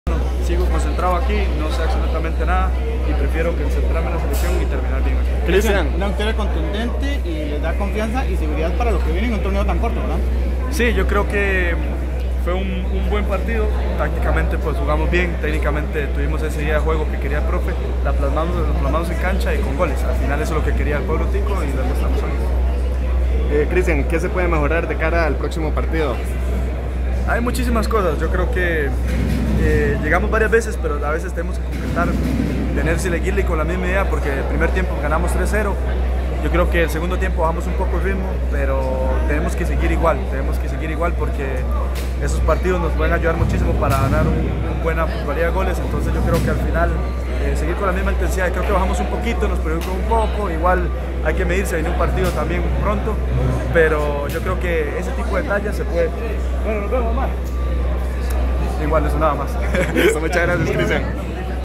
Cristian Gamboa en zona mixta tras victoria 4 0 vs Nicaragua
Cristian Gamboa respondió algunas consultas de El Observador, luego de la victoria de Costa Rica, 4-0, frente a Nicaragua en la primera fecha del grupo B de la Copa Oro 2019.